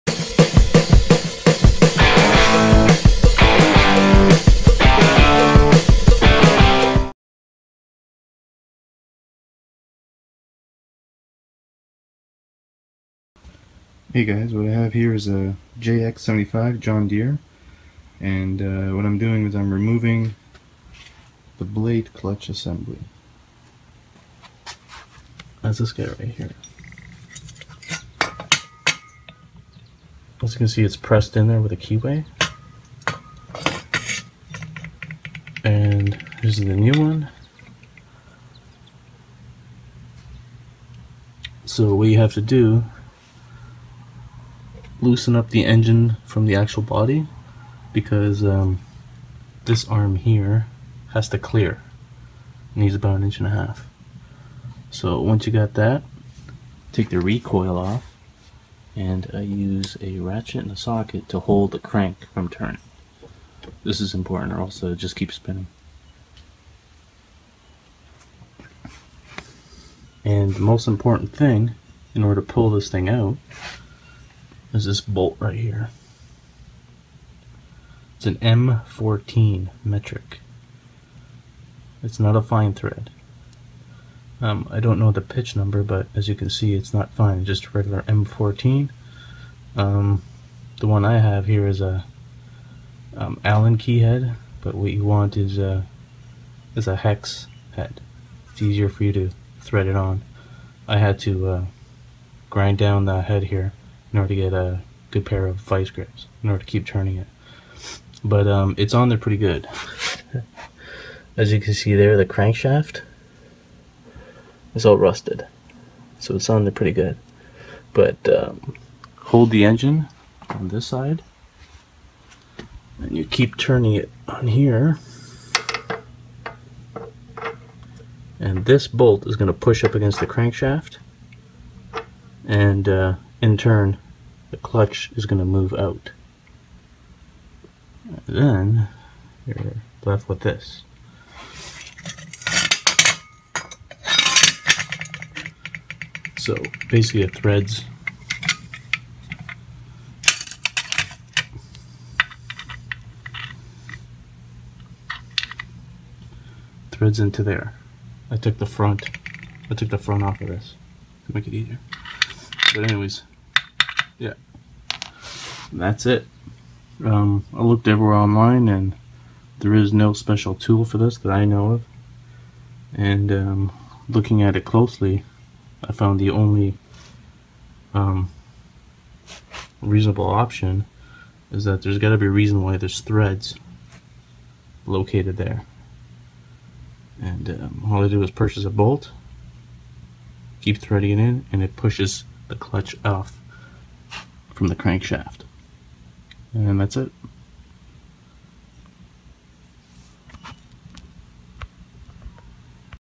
John Deere JX75 Blade Clutch Removal tutorial
In this video I will describe, a procedure I used to remove the blade clutch from a John Deere Jx75.
Sorry, this isn't a visual how-to video like my other videos. But hopefully you will get the just of it.

Thanks.